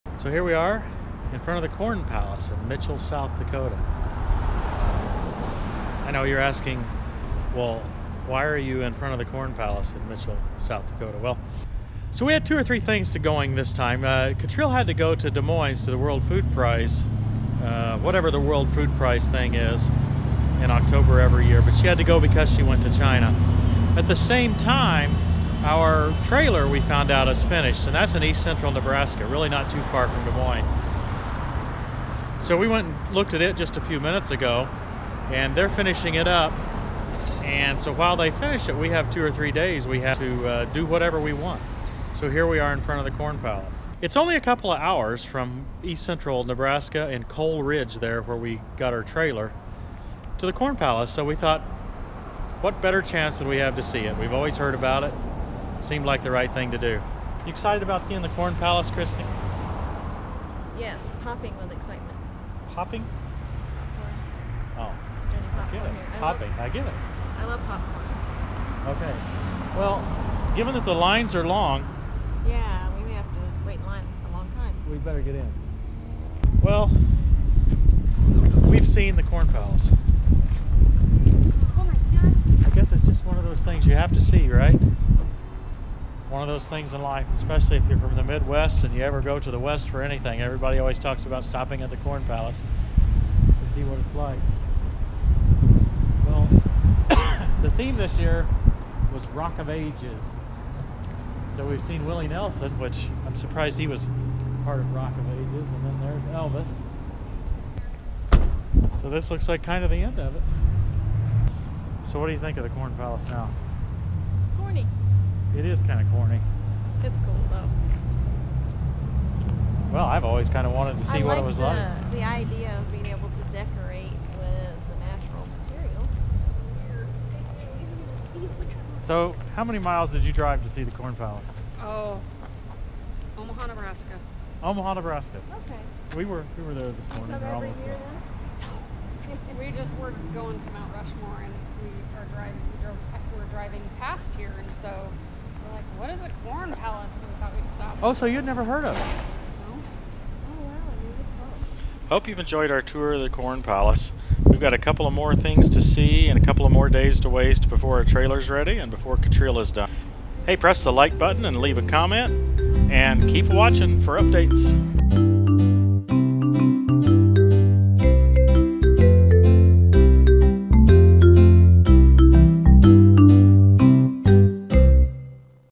#86 Visit the Corn Palace while waiting for our trailer (and Katriel)
Have you ever seen (or heard of) the Corn Palace???
We're on the road to pick up our new trailer.
Since we have a couple of days, we thought we would enjoy some of the local (or somewhat local) sites.

We thought maybe you would enjoy visiting it with us!